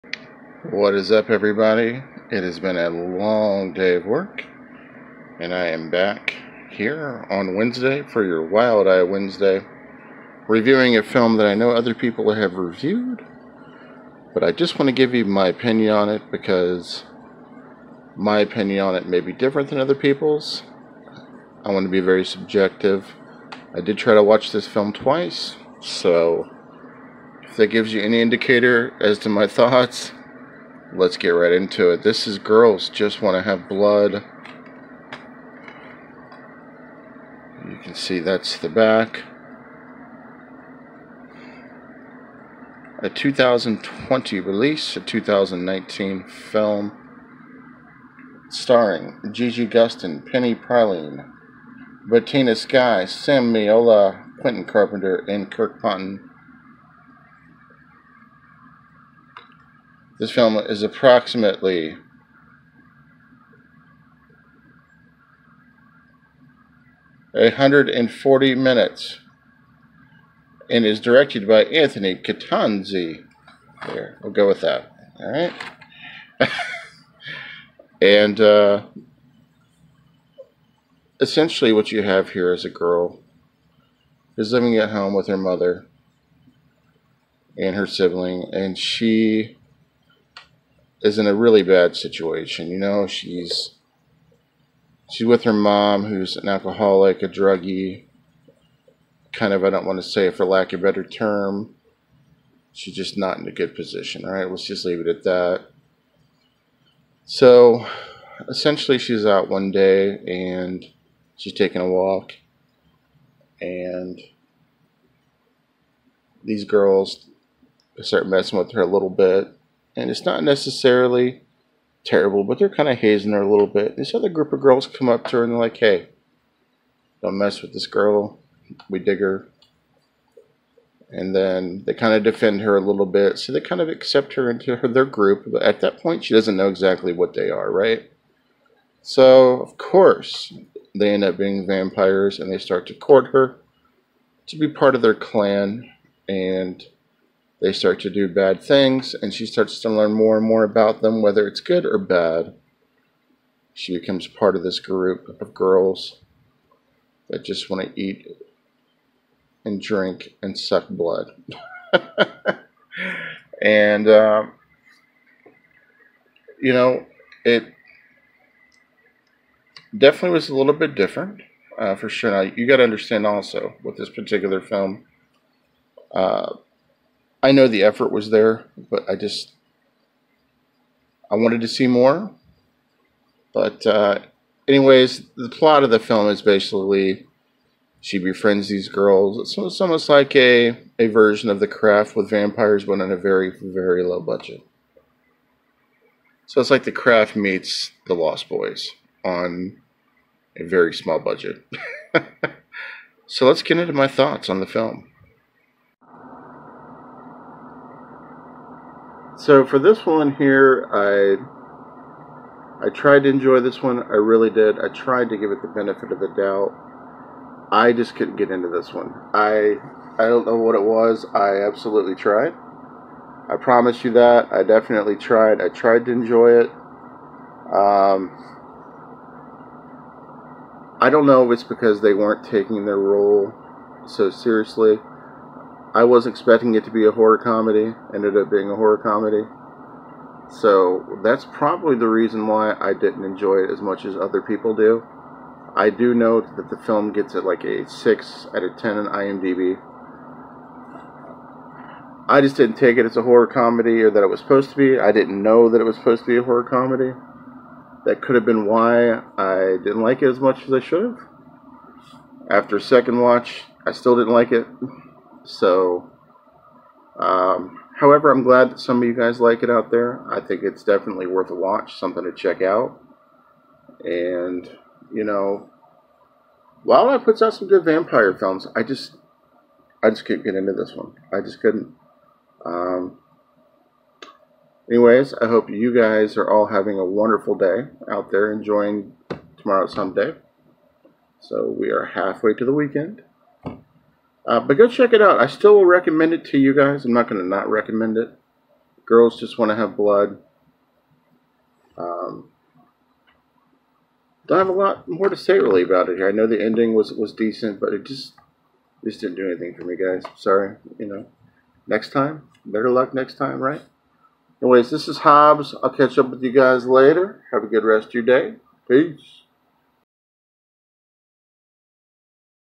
Wild Eye Wednesday "Girls just want to have blood" Review
Today we review the 2019 vampire Indy film Girls just want to have blood!